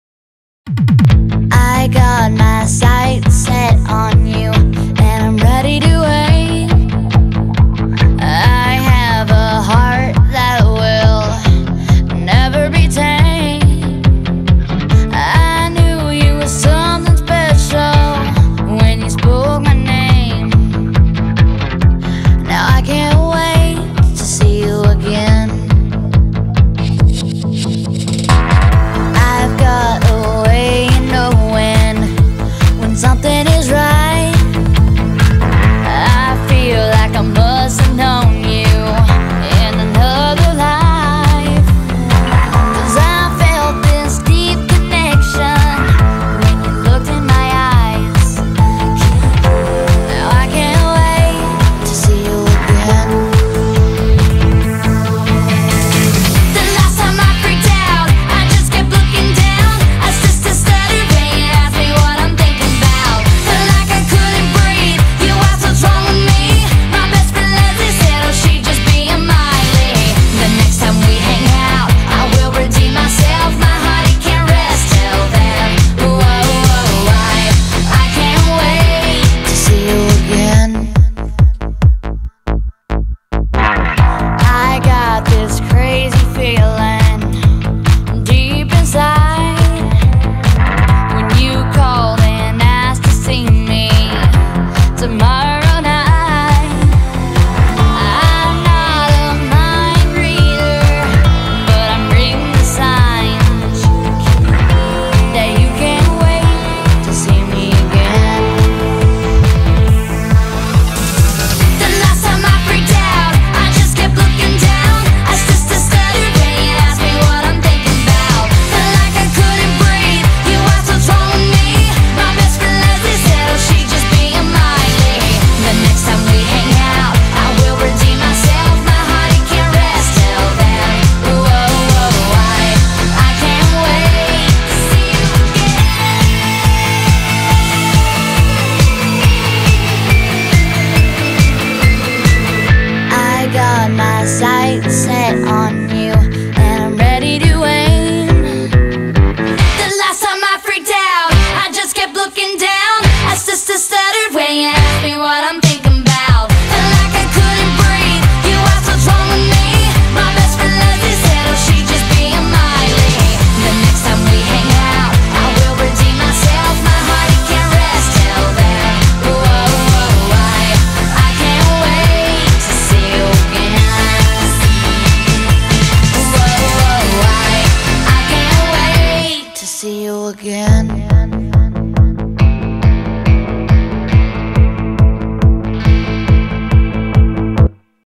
- Niccolò Canepa - Il tributo
Tributo ad uno dei più floridi talenti motociclistici italiani che l'anno prossimo approderà finalmente in MotoGp...